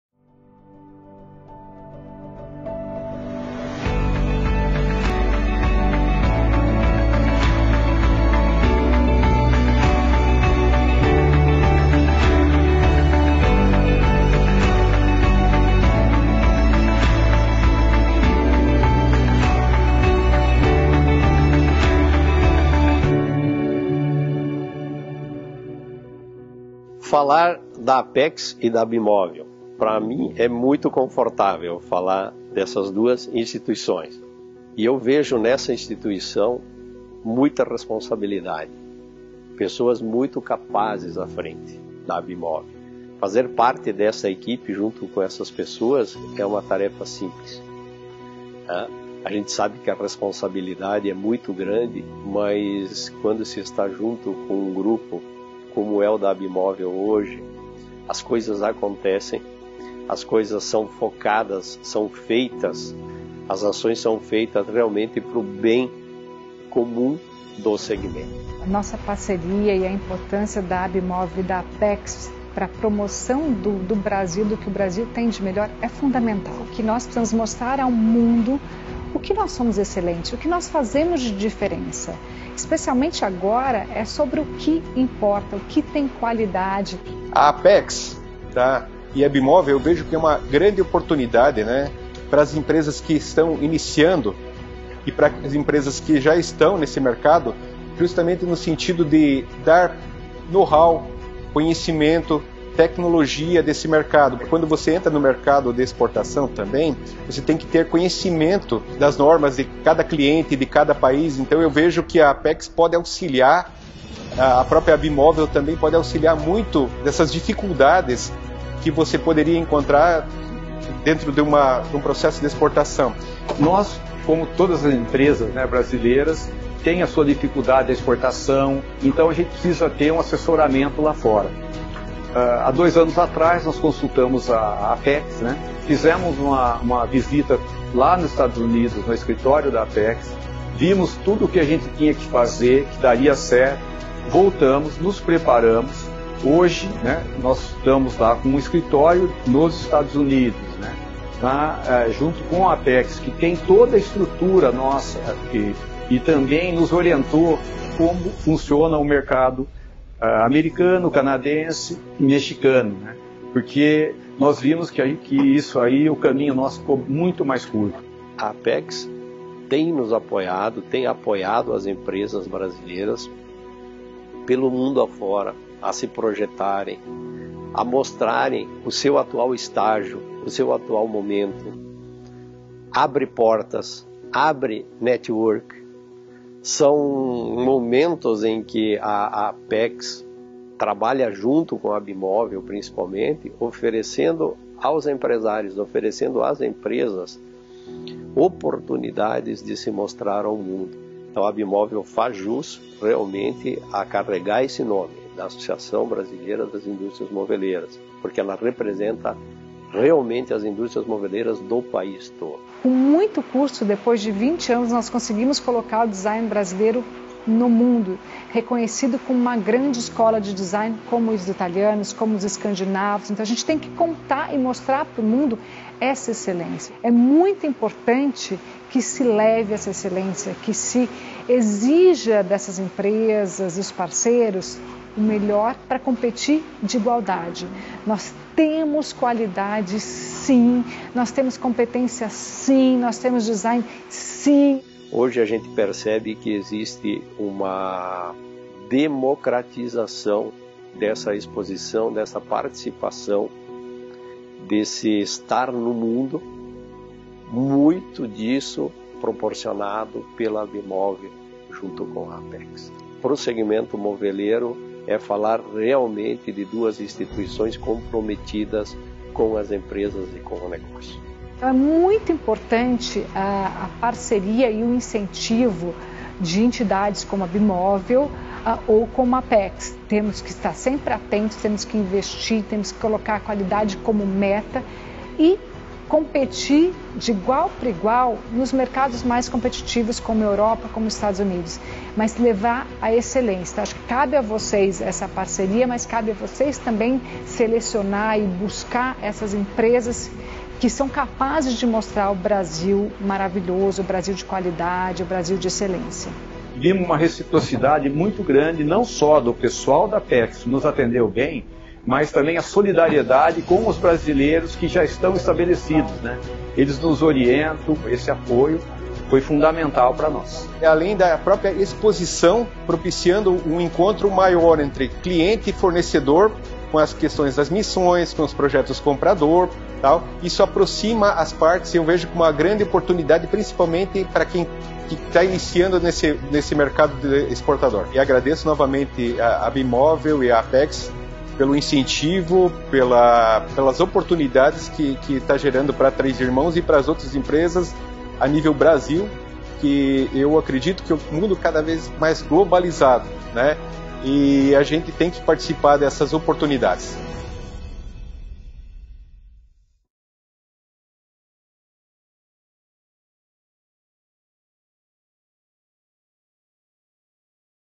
BRAZILIAN FURNITURE - A Indústria Brasileira do Mobiliário - Filme 1 - Móveis o Nosso Negócio
Depoimento das empresas brasileiras associadas ao Projeto Brazilian Furniture, uma parceria entre a ABIMÓVEL - Associação Brasileira das Indústrias do Mobiliário e a Apex-Brasil - Agência Brasileira de Promoção de Exportações e Investimentos.